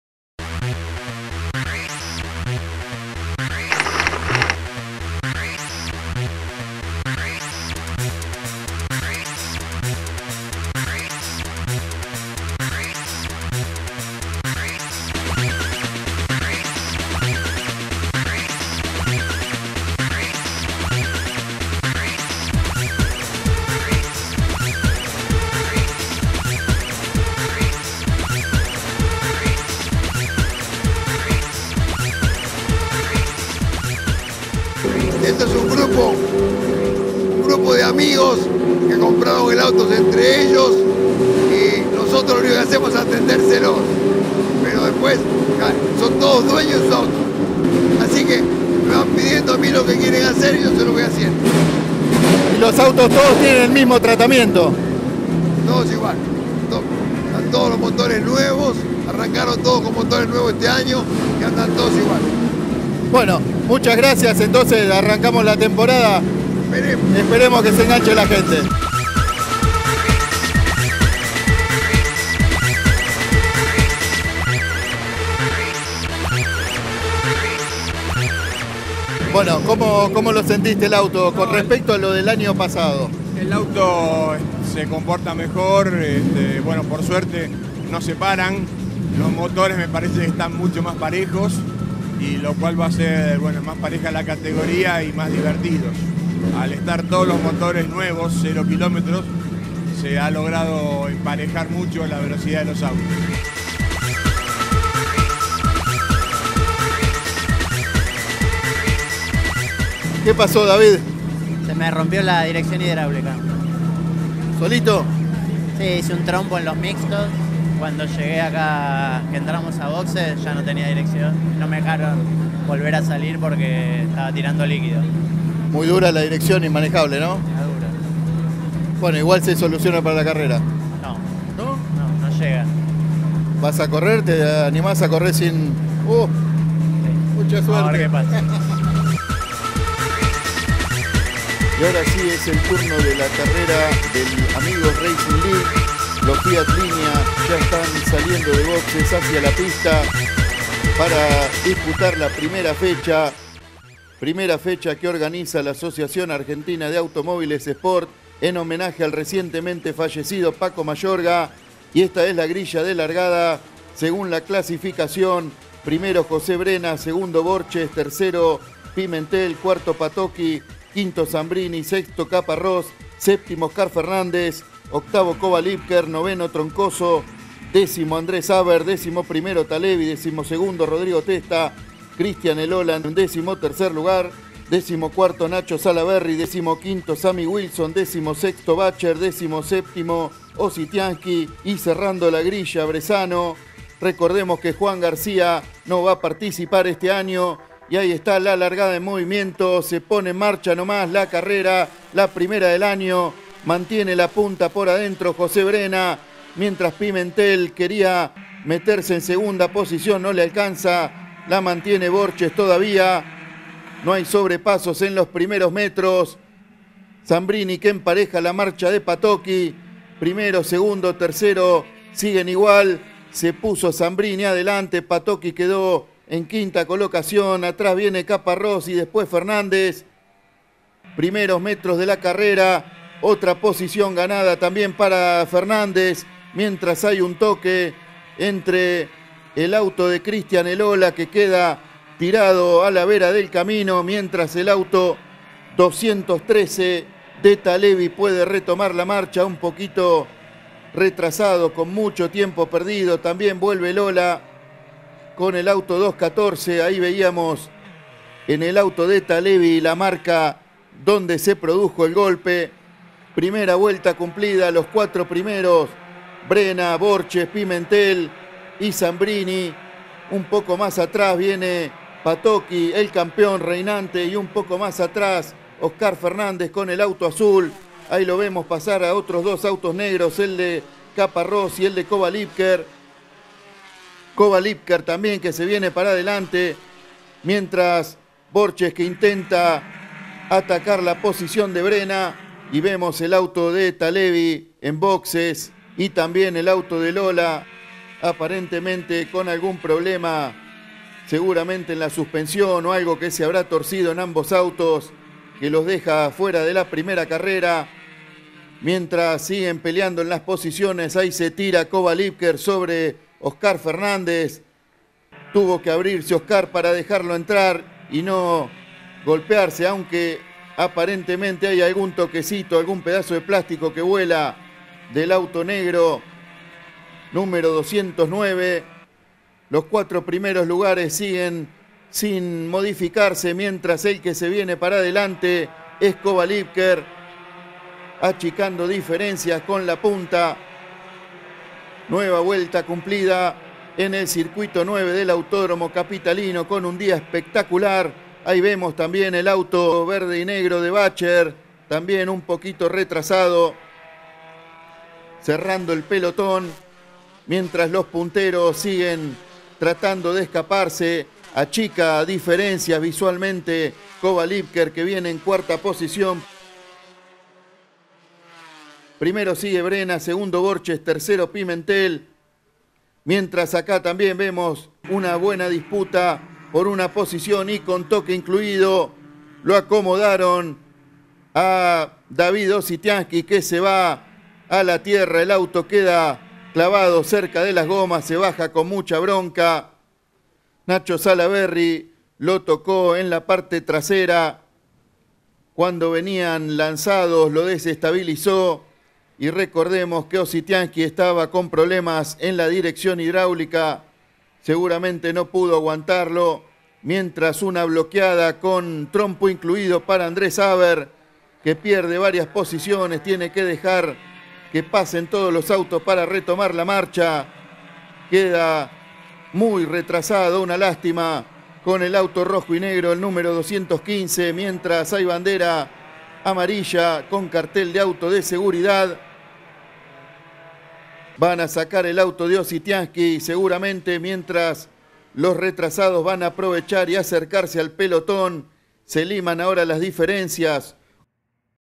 1° FECHA - A.R.L. BS. AS. (1 de 2)
La primera del año de los Fiat Línea, Amigos Racing League!